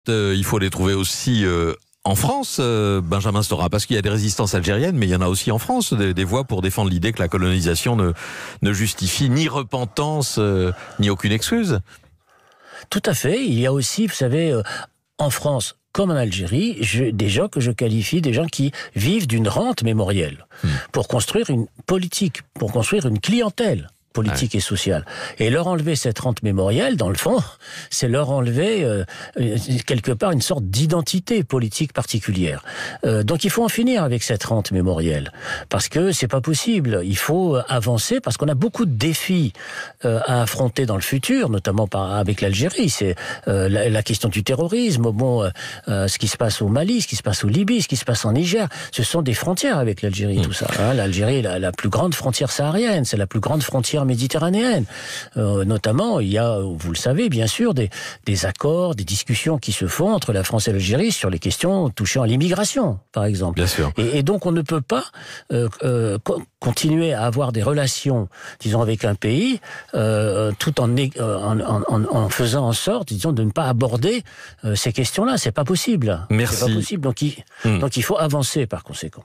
Algérie : Benjamin Stora dénonce ceux qui vivent d'une '"rente mémorielle"
L'historien français Benjamin Stora, auteur d'un rapport sur la colonisation et la guerre d'Algérie, a dénoncé au micro d'Europe 1 les personnes qui vivent "d'une rente mémorielle" en France comme en Algérie. Pour lui il faut "en finir avec ça pour pouvoir avancer".